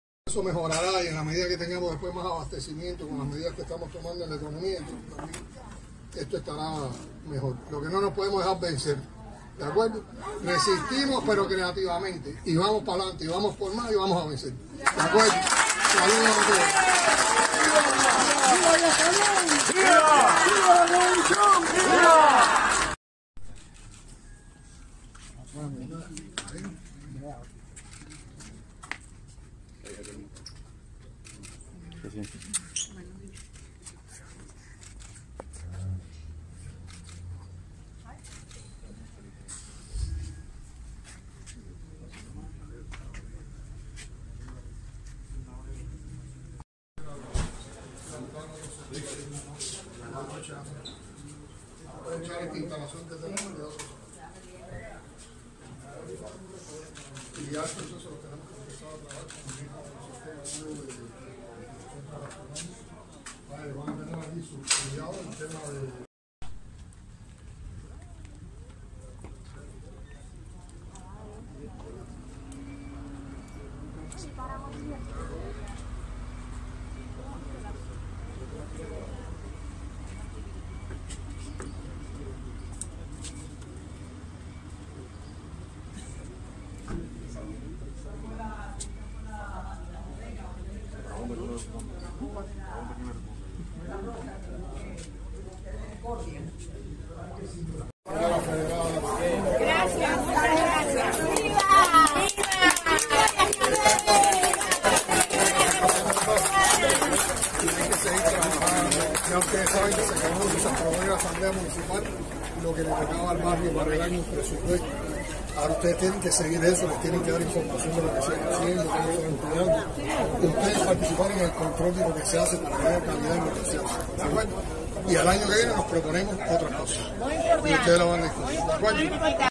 Díaz-Canel a cubanos: '¡No nos podemos dejar vencer! Resistimos creativamente'
Cuando la crisis económica tiene casi asfixiados y cansados a los ciudadanos, el Partido Comunista de Cuba (PCC) planifica a cada rato una visita del presidente designado, Miguel Díaz-Canel, a algún "barrio vulnerable". El dirigente repitió este martes sus habituales consignas a los vecinos del barrio "La Concepción", en el municipio habanero de La Lisa.

"Eso mejorará y en la media en la que tengamos después más abastecimiento con las medidas que estamos tomando en la economía, esto estará mejor. Lo que no nos podemos dejar vencer. ¿De acuerdo? Resistimos pero creativamente y vamos pa´lante y vamos a por más y vamos a vencer. ¿De acuerdo?", dijo desde el mercado La Concepción.

El influencer Magdiel Castro compartió el video y dijo: "Hay que tener poca vergüenza para pedir resistencia con apagones de hasta 18 horas y con la economía al borde del colapso... esto mientras sus familias disfrutan una vida de lujos prohibida para la mayoría de los cubanos".

Otros cubanos opinaron que "la economía cayó por el borde hace bastante, vivimos a base de donativos...", "que cansada me tiene el temita de 'Vamos a vencer', no tienen cara", "cada día son menos, ni ganas de gritar tienen de tanta hambre que hay" y "¿cuánto tiempo llevamos esperando la victoria? ¿Cuando dirán: vencimos? Ya va siendo hora de que cambien las conjugaciones".

Tras las protestas del 11 de julio de 2021, el régimen de La Habana comenzó con su programa de "rehabilitación de barrios vulnerables" en el país.